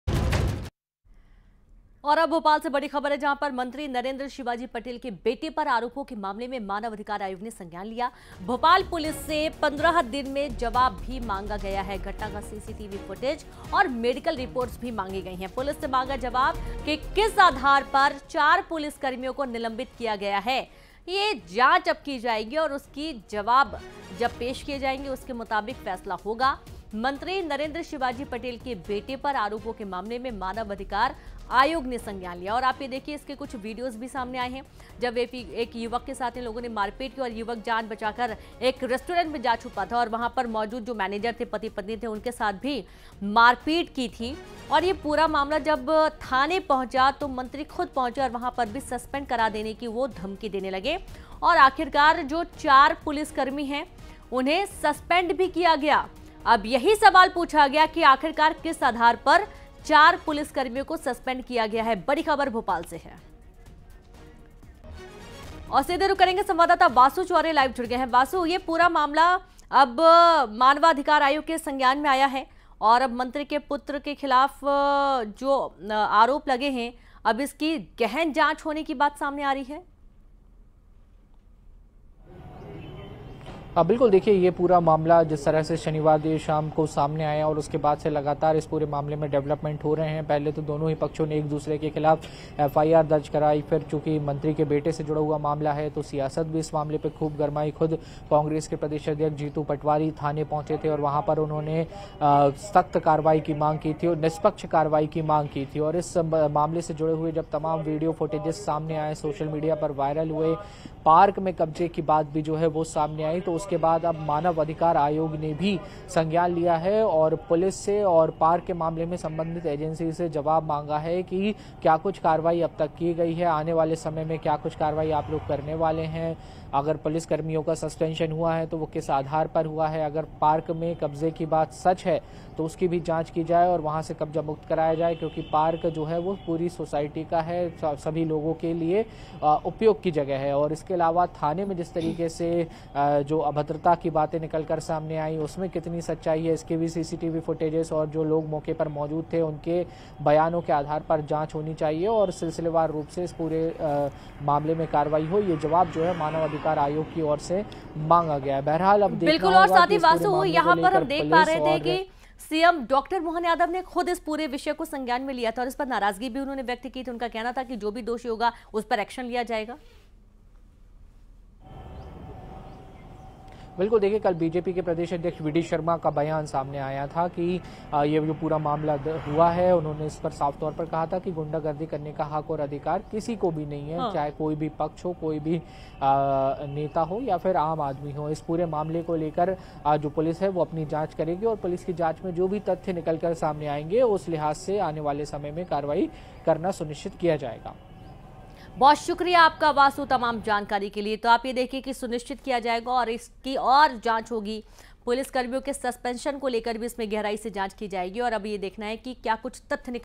Bhopal News : Abhigyan patel की गुंडागर्दी पर गरमाई सियासत | Narendra Shivaji Patel | Jitu Patwari
Bhopal से एक बड़ी खबर जहां Abhigyan patel की गुंडागर्दी पर गरमाई सियासत. इस मामले में अब हो चुकी है मानव अधिकार आयोग की एंट्री.